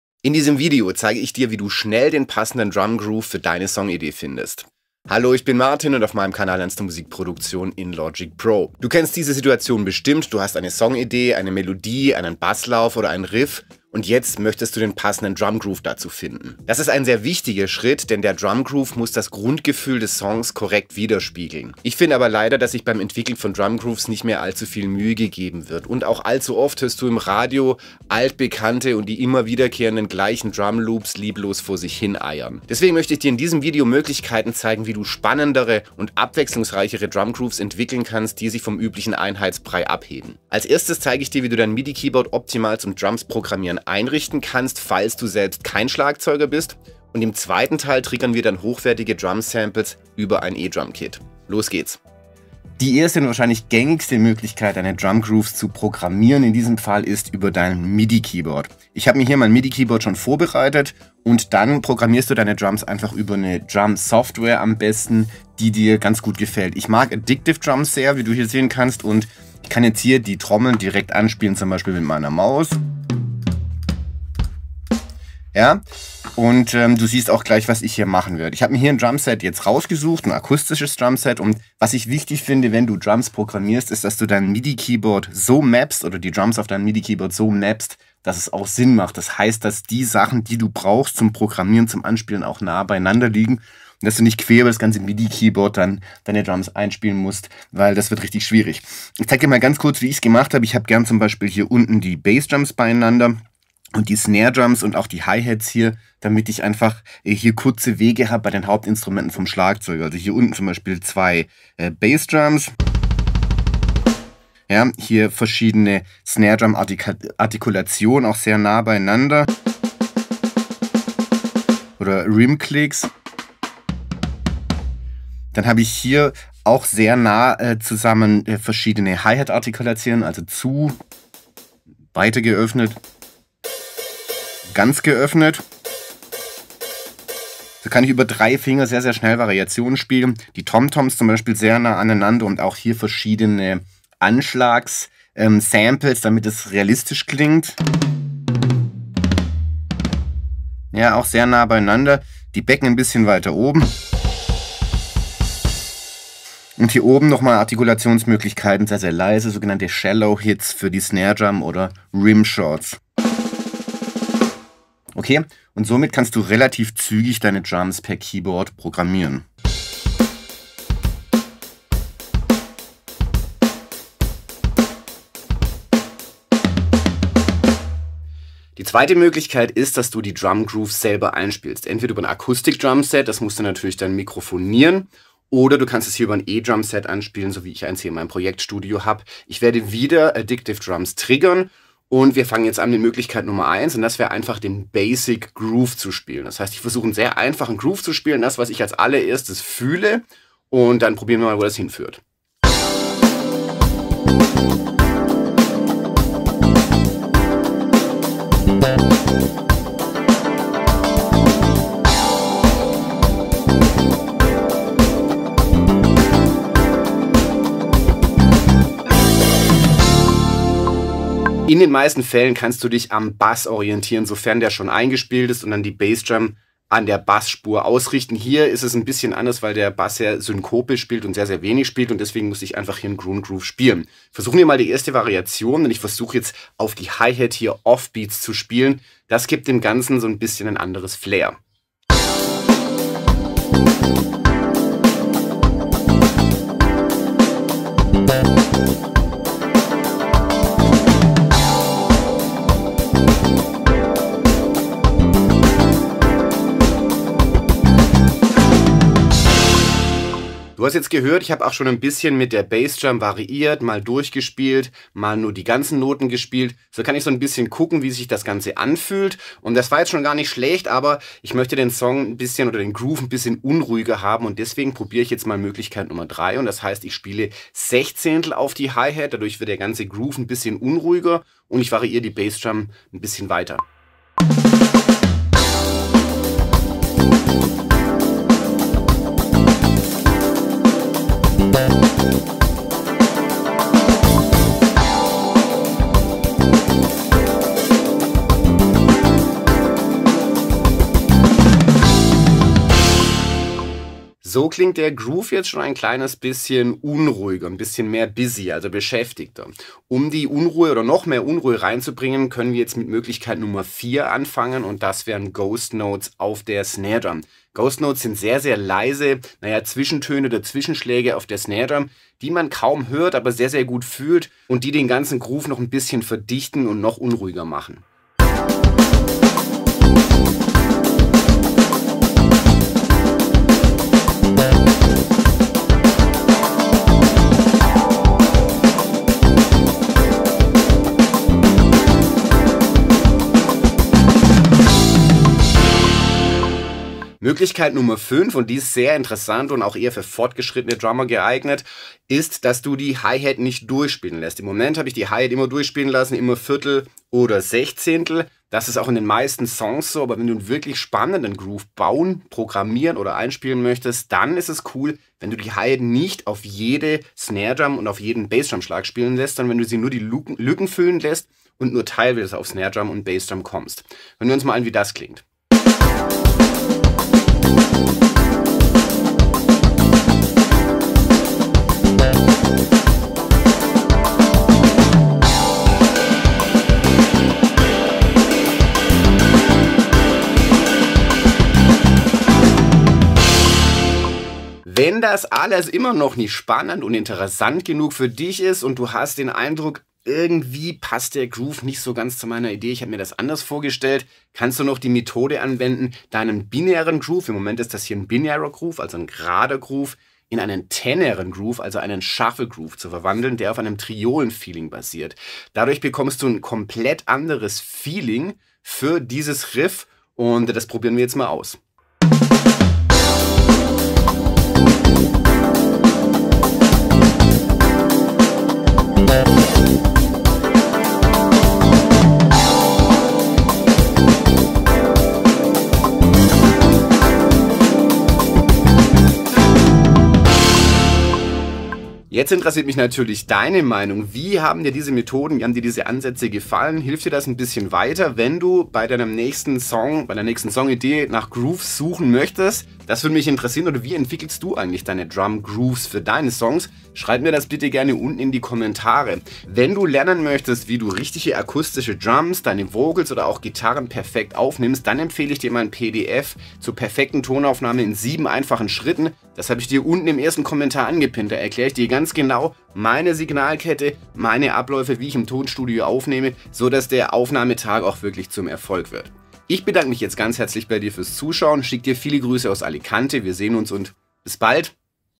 Perfekten Drum Groove finden! (Song Schreiben, Aufbau)
Beim Musik machen fragen sich Anfänger oft: Wie schreibe ich Songs, die meinen Zuhörern sofort gefallen? Der Schlüssel dazu ist oft der richtige Drum Groove. Er muss das Feeling des Songs unterstützen und zusammen mit dem Bass das optimale Fundament bilden.In diesem Tutorial gibt dir Martin Wolfinger die passenden Songwriting Tipps zu diesem Thema.